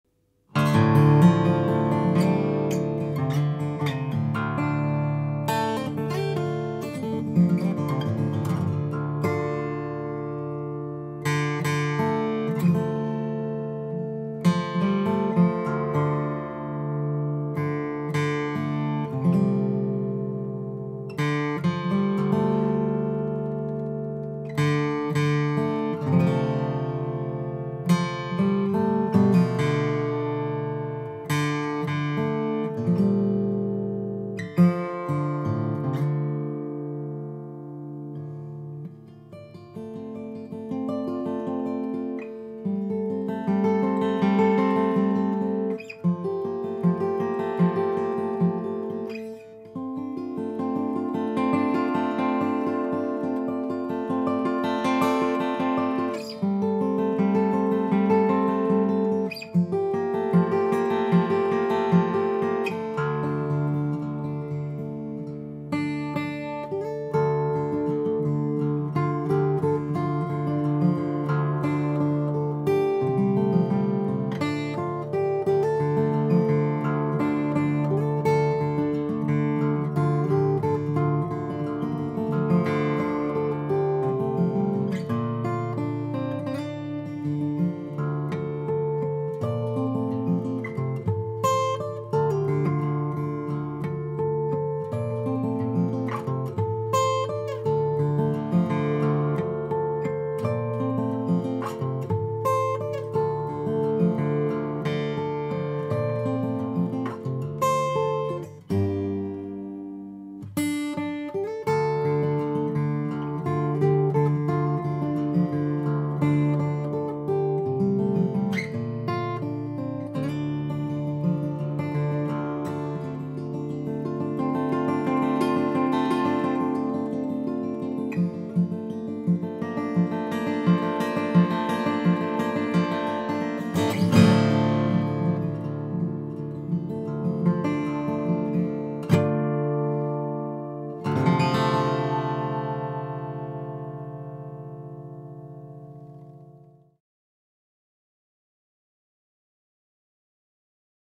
Kostal 00 Acoustic Guitar Played By Stuart Ryan- Part 1
In our humble opinion, Jason Kostal builds one of the best sounding 00 guitars on the planet. How he gets so much tone form such a small-bodied is nothing short of pure wizardry, and this 2017 OO is one of the finest out there!

Built-in a beautiful flitch matched Master Grade Brazilian Rosewood and Master Grade German Spruce this little guitar has a voice as big as a giant. One could describe it as an almost orchestral voice. Truly an absolutely magical guitar.